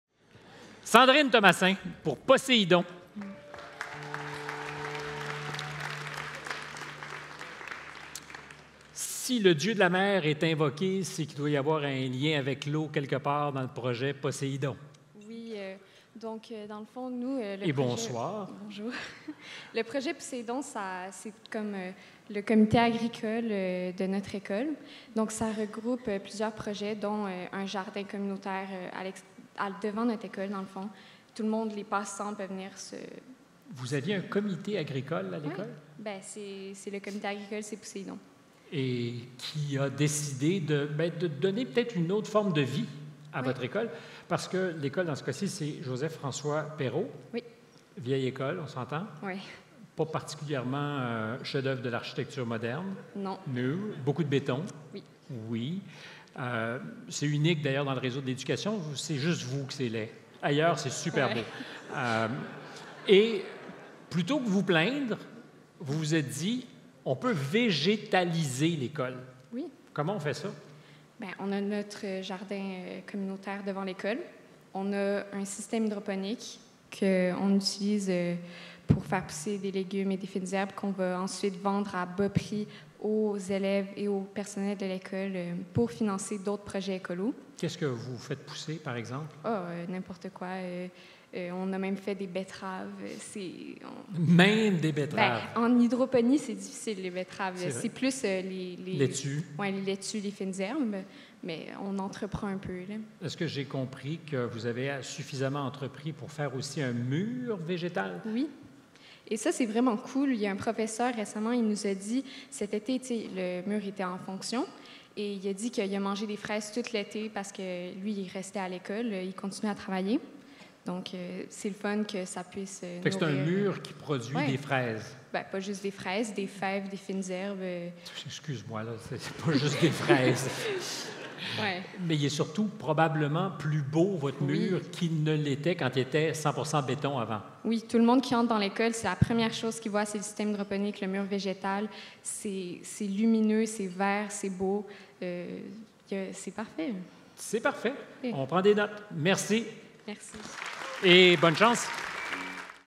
Pousséidon Lauréat AVENIR Projet Engagé Secondaire
Pousséidon, c’est une entreprise d’agriculture intérieure locale et biologique que 3 élèves ont mise sur pied depuis 2 ans pour répondre à des besoins en alimentation saine et pour s’entourer de verdure; de réels défis pour bon nombre de familles vivant dans un milieu urbain bétonné!

En se dotant d’un système hydroponique, ils peuvent cultiver des laitues et autres légumes-feuilles qui égayent la place et purifient l’air avant d’être vendus à bas prix, même l’hiver. Grâce à un financement obtenu dans le cadre d’un concours d’entrepreneuriat, ils ont installé un mur végétal, ce qui leur permet de cultiver d’autres légumes en terre, en partant leurs propres semis. Concrètement, en 1 an, ils ont vendu plus de 300 produits frais, à 3 ou 4 $ chacun.

Production, gestion financière, récolte, vente : cette expérience novatrice permet à tous de savoir que l’on peut cultiver nos propres légumes et avoir une certaine autonomie alimentaire en tout temps!